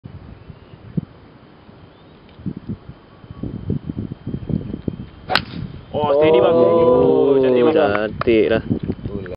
Mardan Mamat Tee Shot at CIMB Asia Pacific Classic Malaysia 2010
This was on the 6th tee box at Mines Resort & Golf Club during the CIMB Asia Pacific Classic Malaysia 2010. It was recorded using Nikon D90.